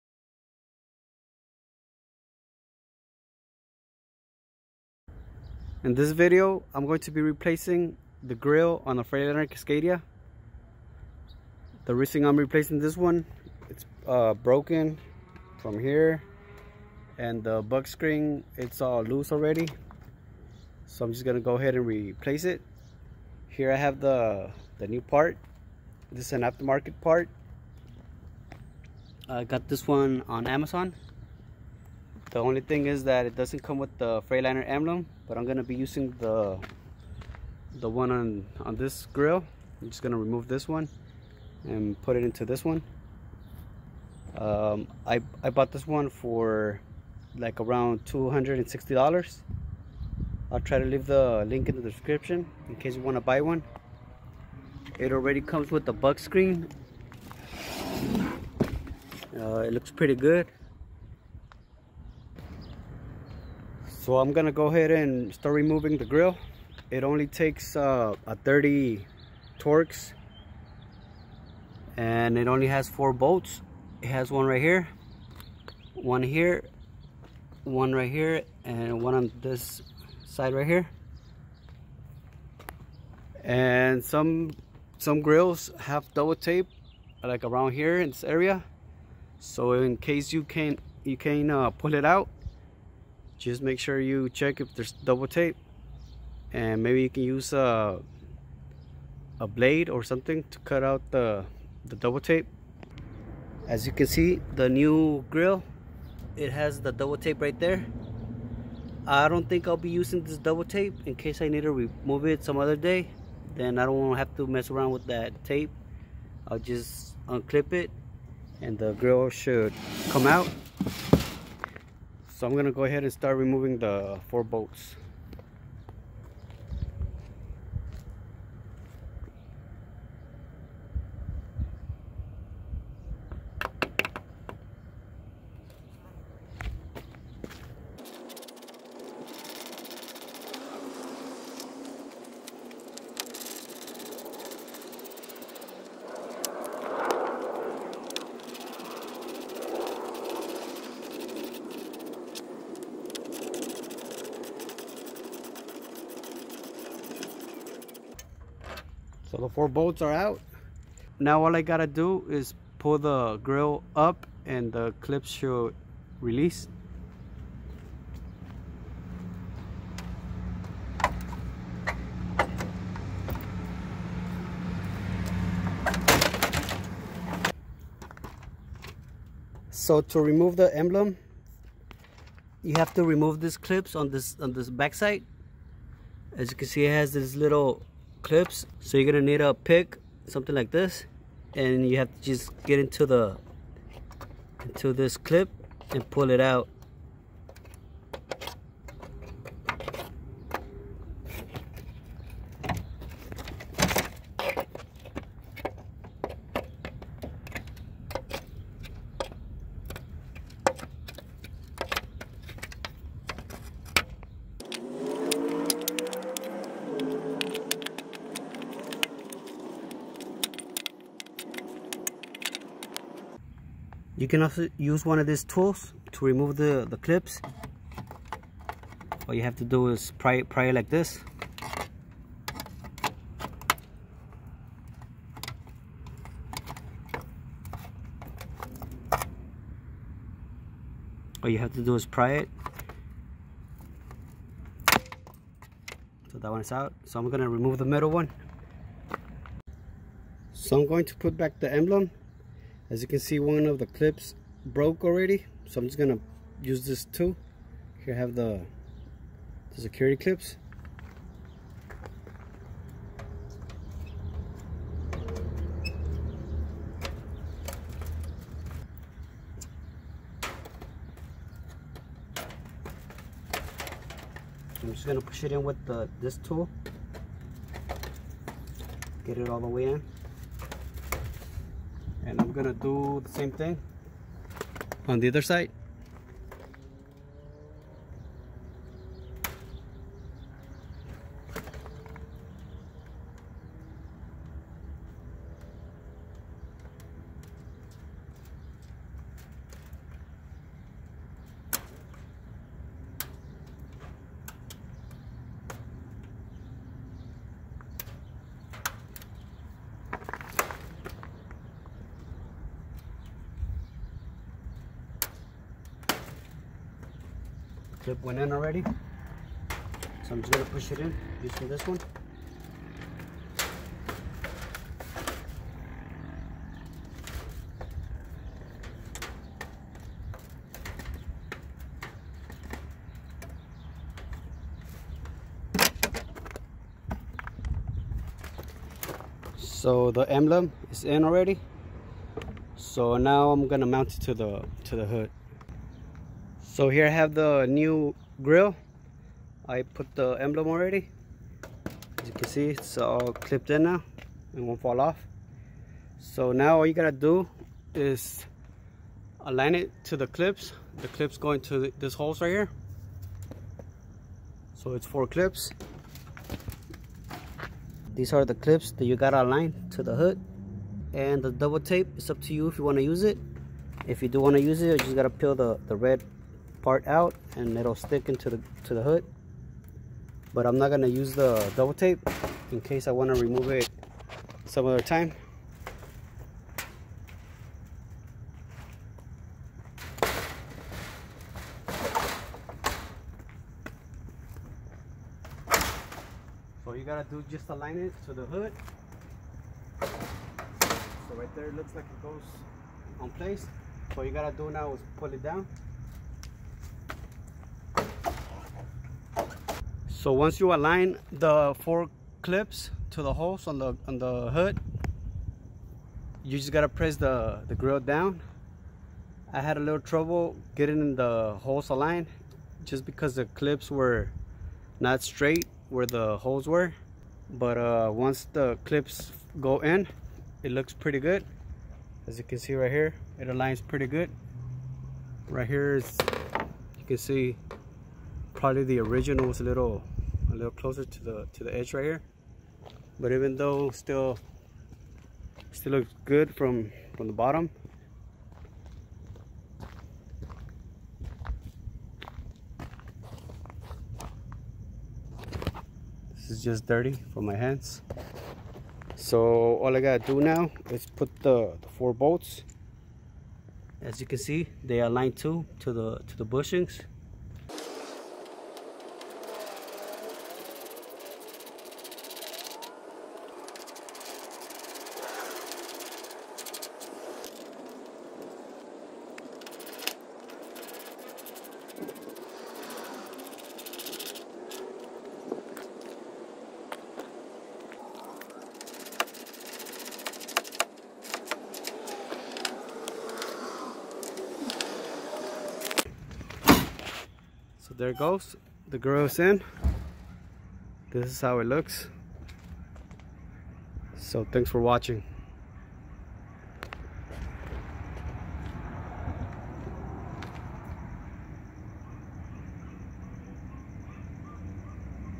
How to remove and replace grill on freightliner cascadia
For Freightliner Cascadia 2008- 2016  Chrome Triple Plated Front Hood Radiator Grill Grille With Bug Screen A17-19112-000 A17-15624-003